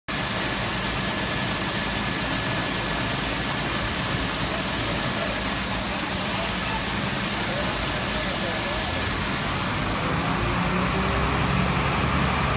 Grote brand in Palazzo Theater in Grave
Beelden van grote brand in Theater Palazzo in Grave.